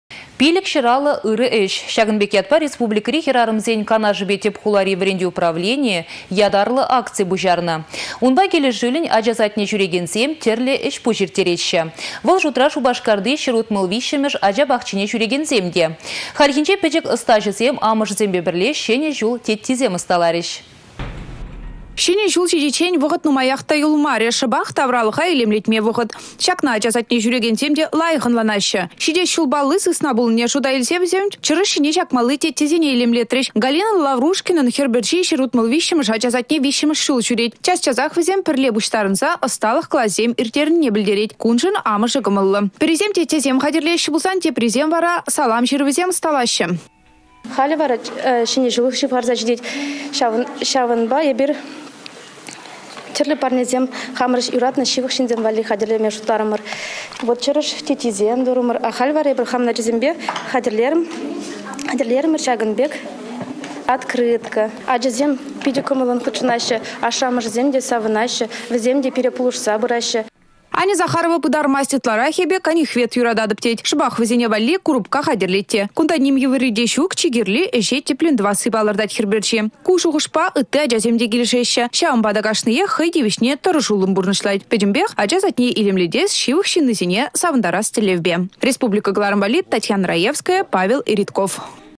550 ырă ĕç
550 ырă ĕç — çакăн пек ятпа Республикăри хĕрарăмсен канашĕпе тĕп хулари вĕрентӳ управленийĕ ятарлă акци пуçарнă. Унпа килĕшӳллĕн ача садне çӳрекенсем тĕрлĕ ĕç-пуç ирттереççĕ. Вăл шутра - Шупашкарти 163-мĕш ача пахчине çӳрекенсем те. Хальхинче пĕчĕк ăстаçăсем амăшĕсемпе пĕрле çĕнĕ çул теттисем ăсталарĕç.